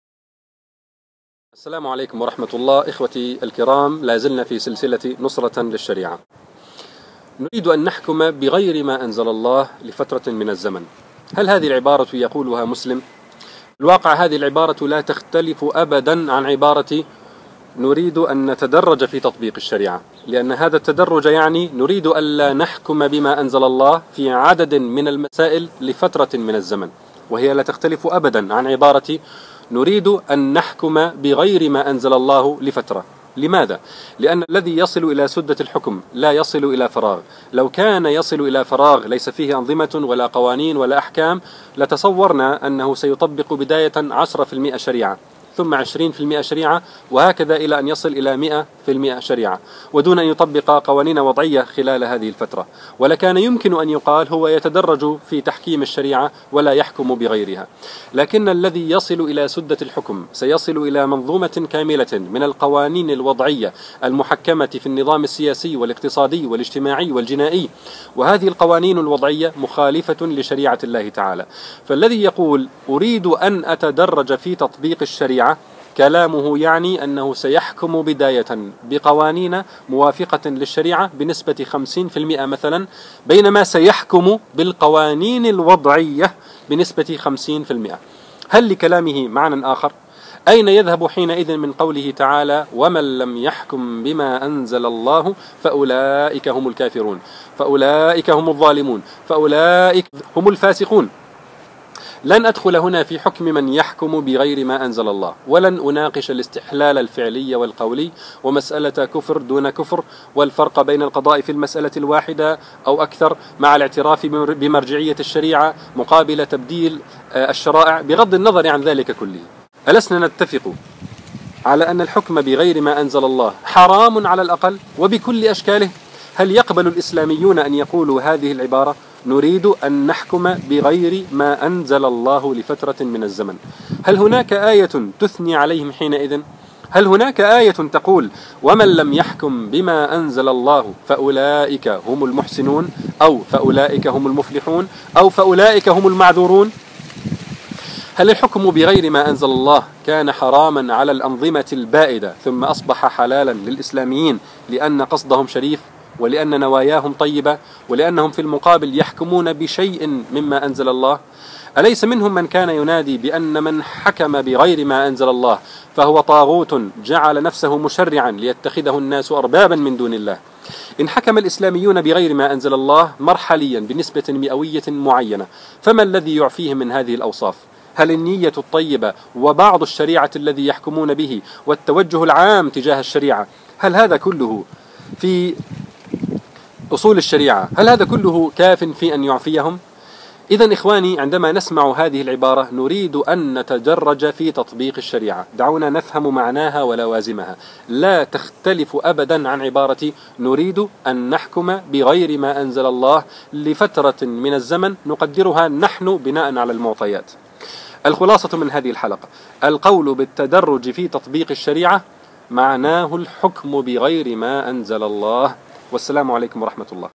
نصرة للشريعة ٣: المعنى الخطير للتدرج في تطبيق الشريعة
التدرج في تطبيق الشريعة بالمفهوم المتداول = نريد أن نحكم بغير ما أنزل الله لفترة من الزمن. لا معنى له غير ذلك!